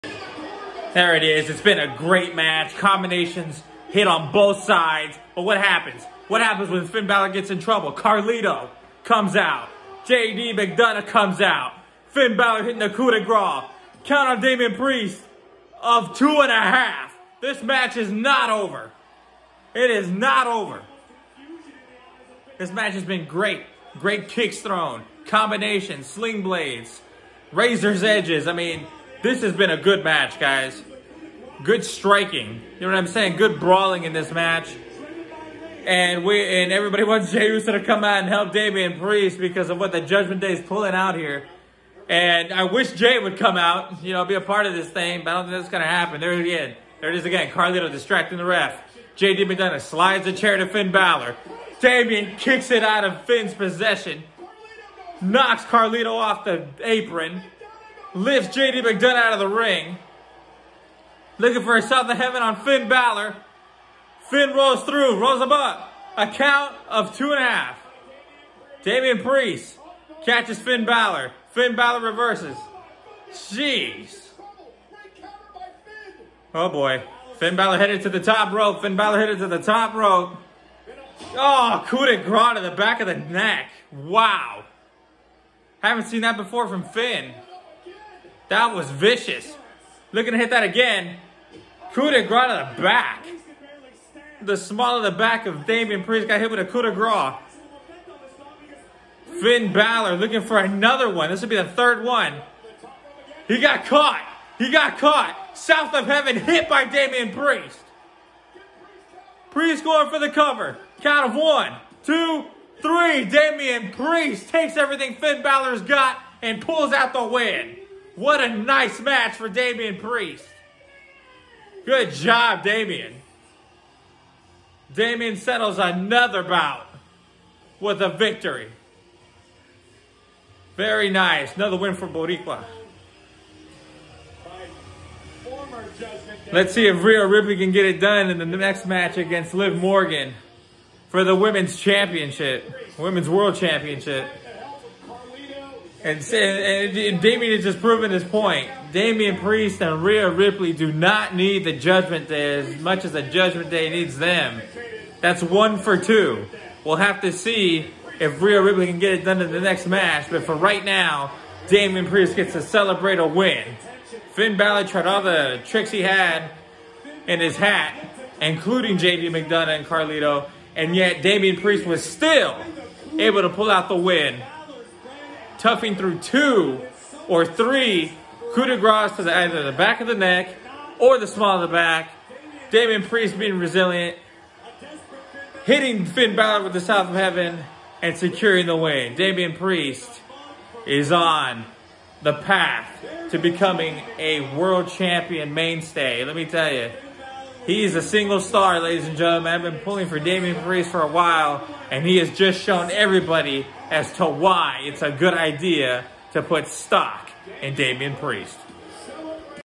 Damian Priest topples Finn Balor at Bad Blood
Damian Priest Has topped Finn Balor and the judgment day despite their antics